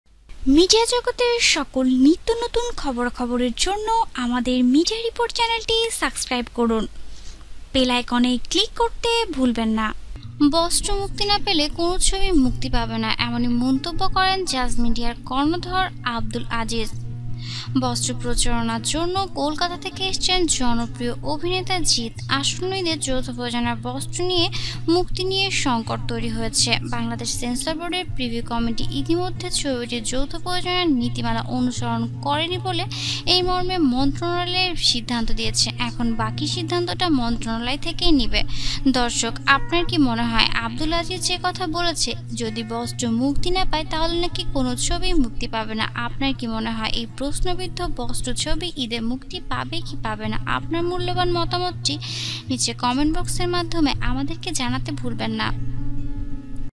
ঈদে বস টু মুক্তি না পেলে কোনো ছবিই মুক্তি পাবে না | বস টু সম্মন্ধে এ কি বললো জিৎ ও নুসরাত ফারিহা
►► ঝড় তুলেছে শাকিব বুবলীর রোমান্স | ঈদ চলে এলো কিন্তু এখনো চলছে ঈদের ছবির শুটিং | শাকিব বাবলির রংবাজ